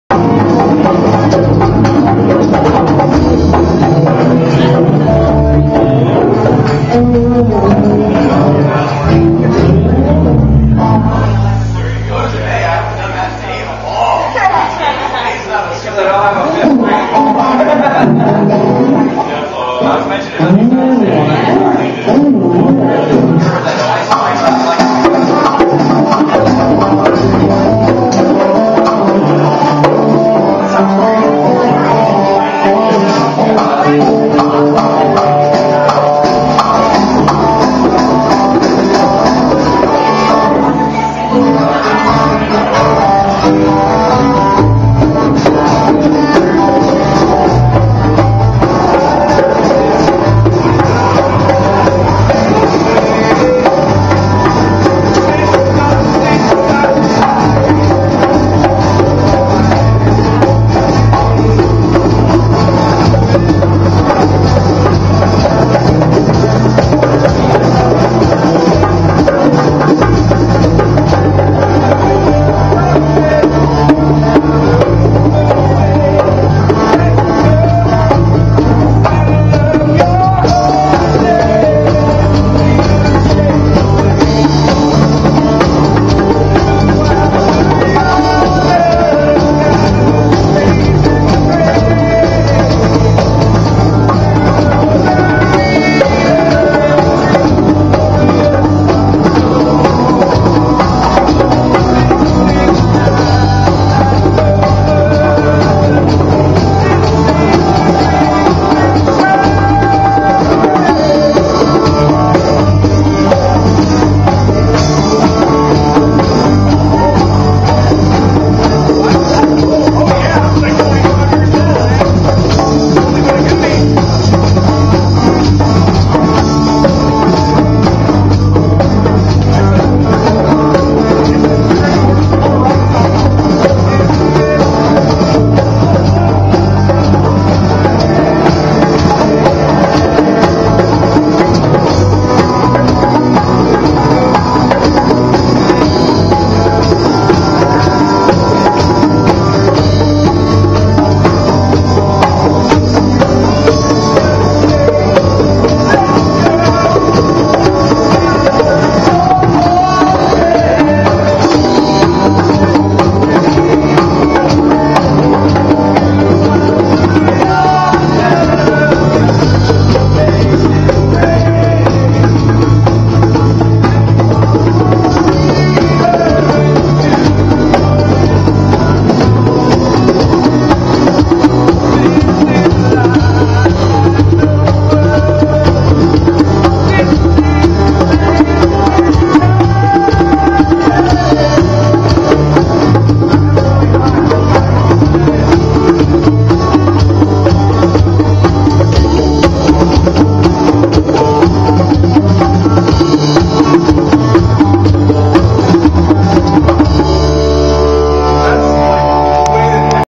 SEVENTH DAY ADVENTISTS GETTING DOWN IN MERDIAN IDAHO AFTER CHURCH
THE BAND PLAYS AROUND AFTER CHURCH